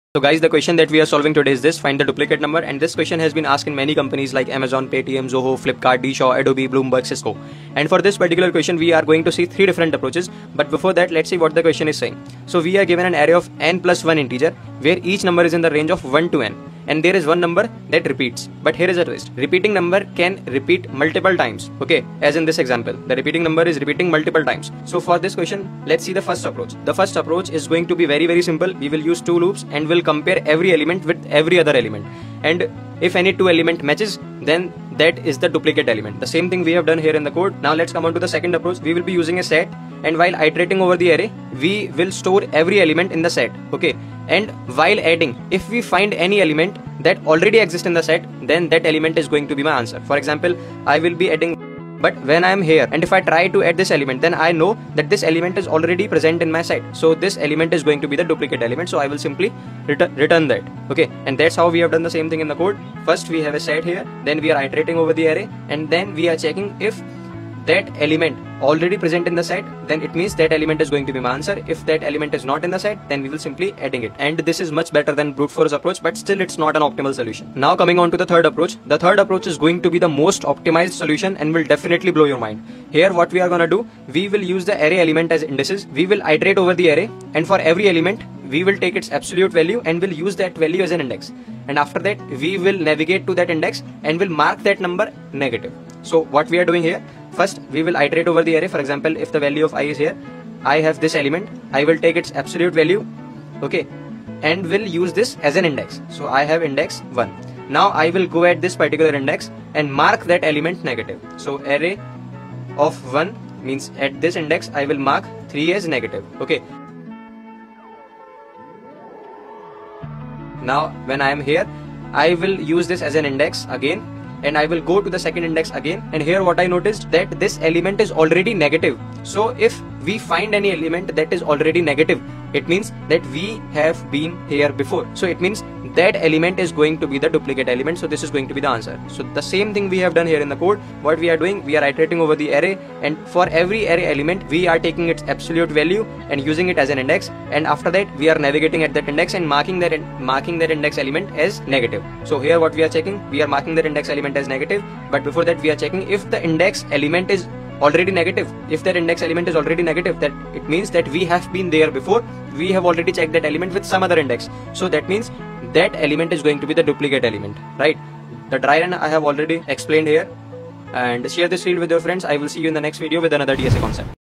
LeetCode 287 | Find The Duplicate Number
This is one of those tricky questions that most people get wrong.

We are given an array containing n + 1 integers where each integer is in the range 1 to n

And we have to return the duplicate number but here’s the twist, the number can repeat more than once.

leetcode for beginners, coding, coding with yash, codingwithyash, leetcode, leetcode problems, find duplicate number, leetcode 287